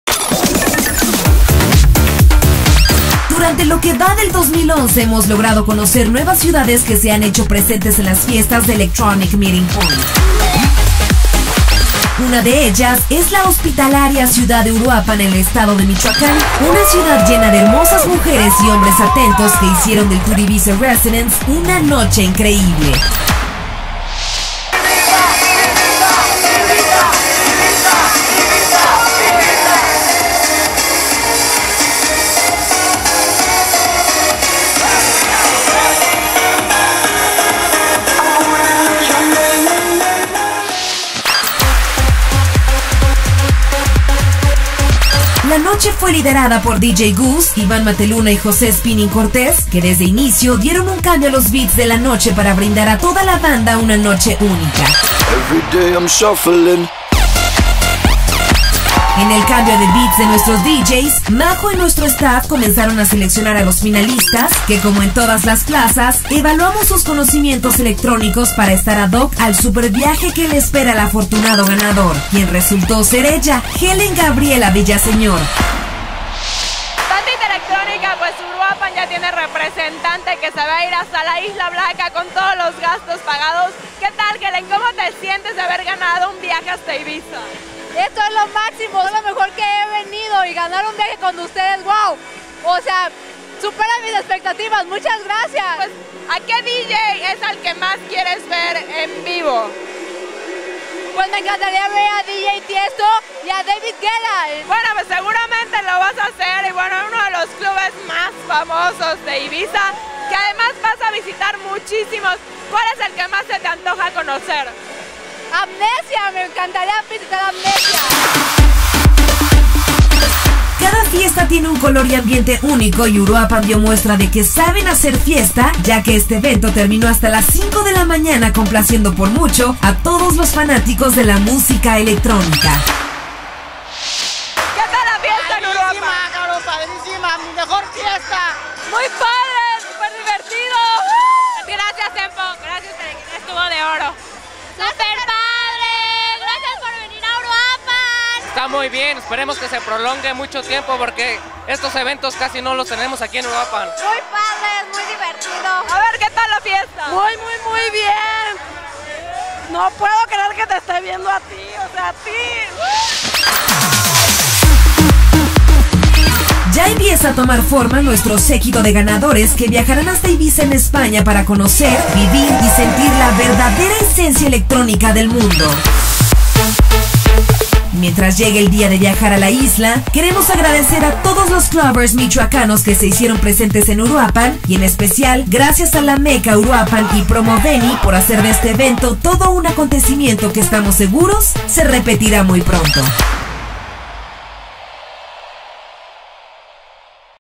Reporte Ibiza Residence 2011 Uruapan
Tour Ibiza Residence 2011 llego a Uruapan, Michoacan.
Ganadora del viaje a Ibiza Hellen Gabriela Villaseñor Rodriguez!